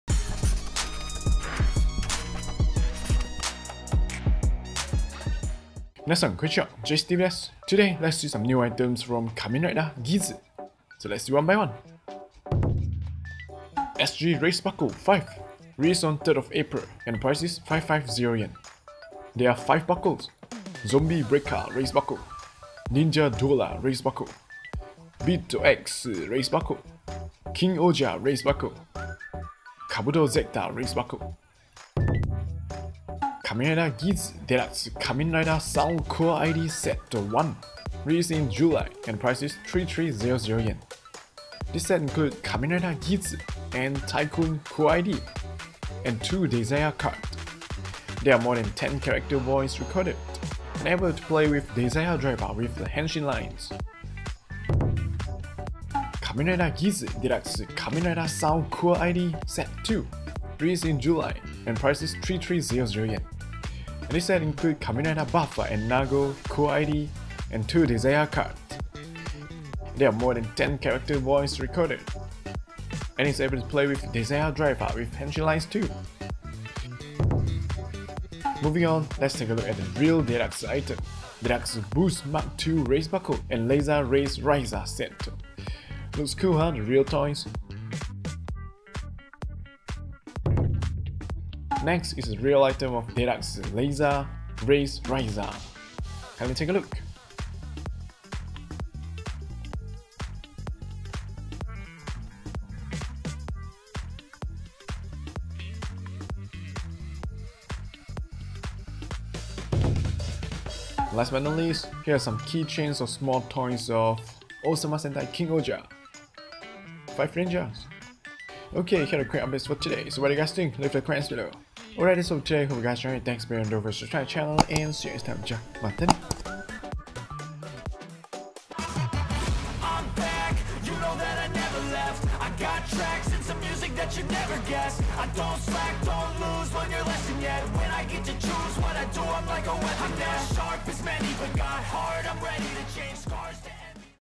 【仮面ライダーギーツ】新SGレイズバックル5！DX仮面ライダーサウンドコアIDセット01/02！New SG Raise Buckle 5！DX Kamen Rider Sound Core ID！
SGレイズバックル5
SG Raise Buckle 5
発売 Release: 3rd April (4月3日)
価格 Price: 550円JPY x1

●連動アイテム（全5種）
1．ゾンビブレイカーレイズバックル
Zombie Breaker
2．ニンジャデュアラーレイズバックル
Ninja Dueler
3．ビートアックスレイズバックル
Beat Axe
4．キングオージャーレイズバックル
King-Ohger
5．カブトゼクターレイズバックル
Kabuto Zector
●ラムネ菓子1個

DX仮面ライダーサウンドコアIDセット」が登場！
01には仮面ライダーギーツ／タイクーン、
02には仮面ライダーナーゴ／バッファを収録♪

仮面ライダーギーツ　DX仮面ライダーサウンドコアIDセット01
Kamen Rider Geats DX Kamen Rider Sound Core ID Set 01
発売 Release: July (7月)
価格 Price: 3,300円JPY
仮面ライダーギーツ／タイクーン
Kamen Rider Geats / Tycoon

仮面ライダーギーツ　DX仮面ライダーサウンドコアIDセット02
Kamen Rider Geats DX Kamen Rider Sound Core ID Set 02
仮面ライダーナーゴ／バッファ
Kamen Rider Nago / Buffa